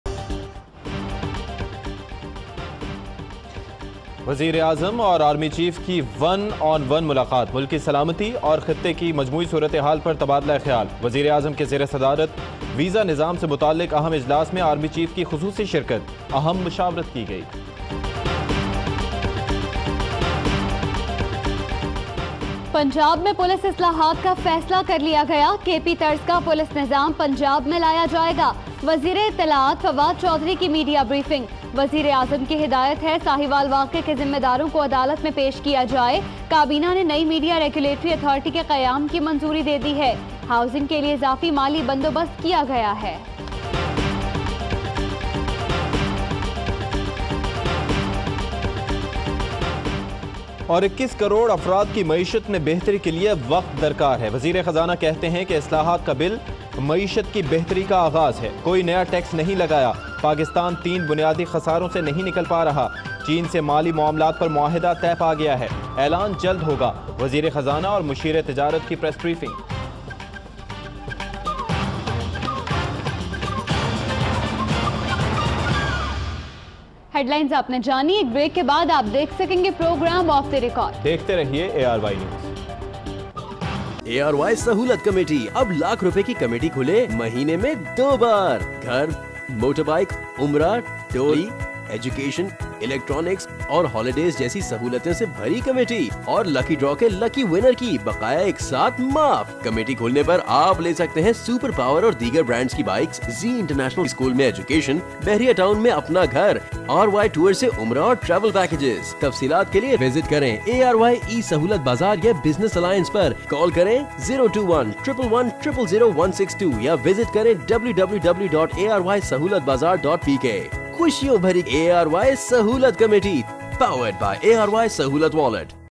Headlines | ARYNews | 2000 | 24 January 2019
Naqeebullah’s murder termed ‘extrajudicial killing’, ATC quashes cases

Pak conducts launch of short range ballistic missile ‘Nasr’

Hamza demands judicial probe into Sahiwal shooting

Rangers arrest man behind national identity cards theft in Landhi

Sindh govt proposes bearing treatment expenses of emergency cases

SA adopts resolution against plans to shift PIA headquarters

Sharif complains of pain in chest, arm

Sheikh Rasheed to inaugurate new freight train in Karachi tomorrow